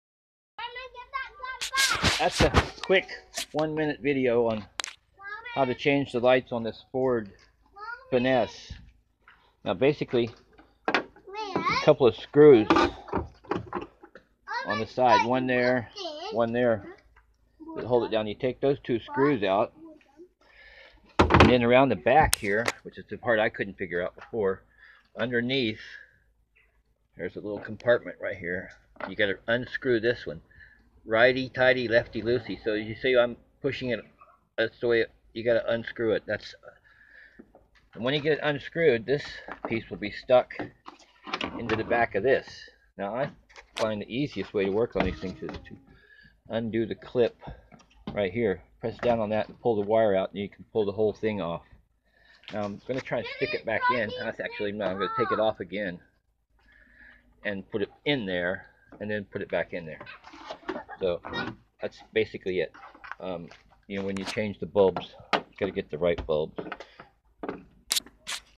Best tip!!! 4 Replacing Brake Lights Ford Fiesta. FLAME
There is one tricky screw   the video shows where it is,,,i am told by viewers   i  have the missing  tip   for   changing  the bulbs,,, working on daughters 700£ 2005 Ford Fiesta Flame   great cheap transport,  she now has a ford  cmax   that i will have to repair